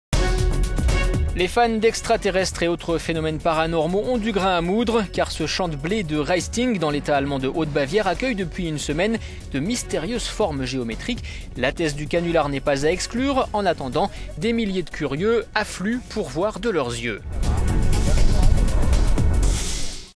De mystérieuses formes géométriques dans un champ allemand
Les fans d'extra-terrestres et autres phénomènes paranormaux ont du grain à moudre. Car ce champ de blé de Raisting, dans l'État allemand de Haute-Bavière, accueille depuis une semaine de mystérieuses formes géométriques. La thèse du canular n'est pas à exclure. En attendant, des milliers de curieux affluent, pour voir de leurs yeux.…